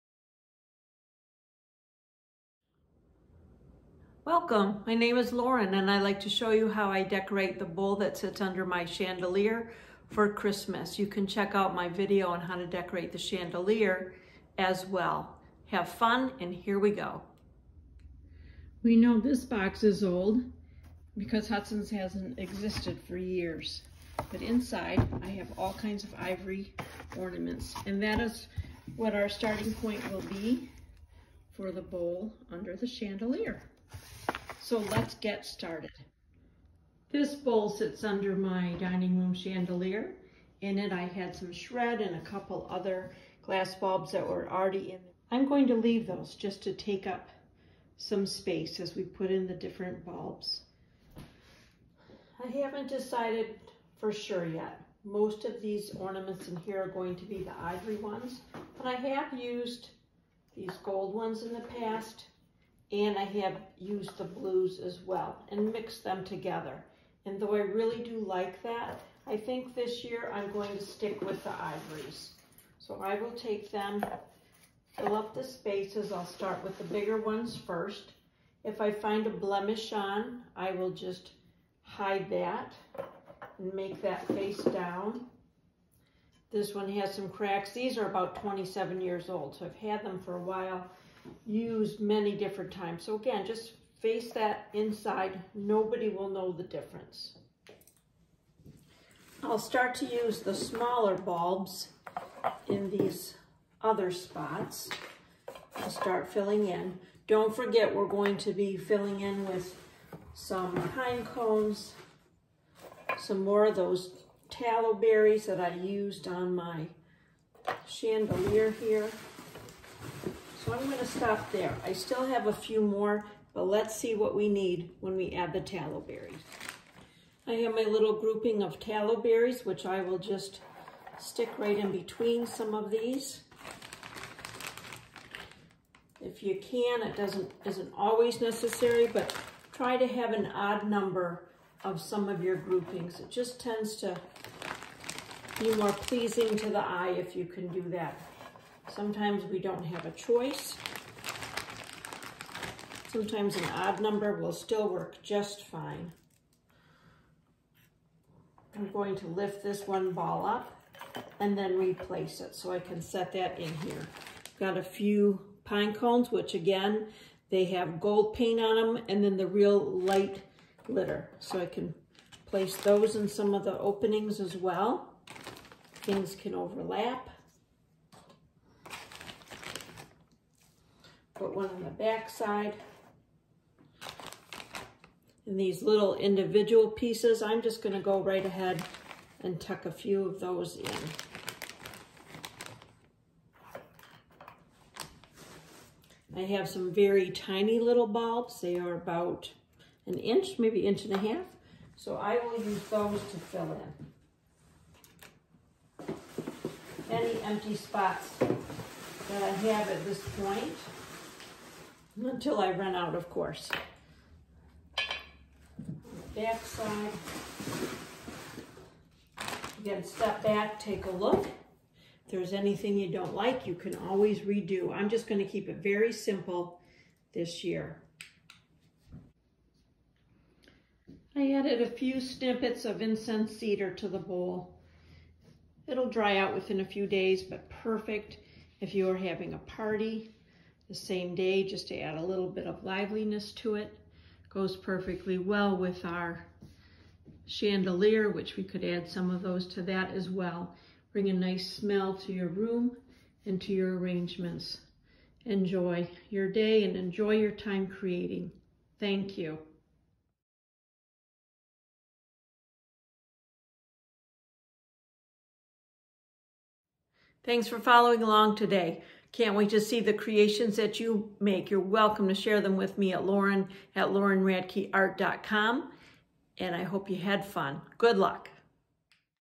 How to Fill a bowl with Christmas Decorations - make it stunning to match chandelier
I will show you how I fill my favorite bowl with Christmas items including Christmas bulbs, glittered pinecones, tallow berries and incensed cedar to create a beautiful centerpiece for your table. This coordinates with my chandelier.

Look for my video on how to decorate your chandelier with similar items.